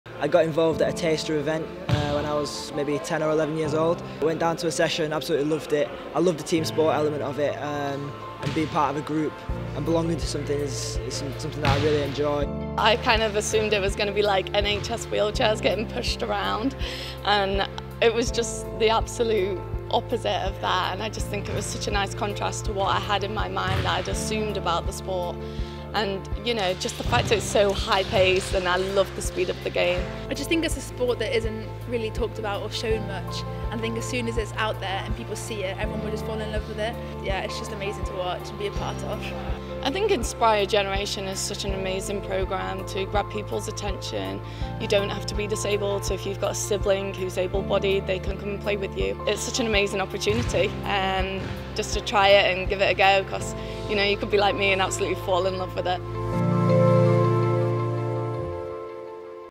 Inspire a Generation
GB National Team Athletes share their own stories about how they found the sport and what wheelchair basketball has meant to them.

To find out about your local clubs Open Events or to sign up to an Inspire a Generation six week wheelchair basketball course in your local community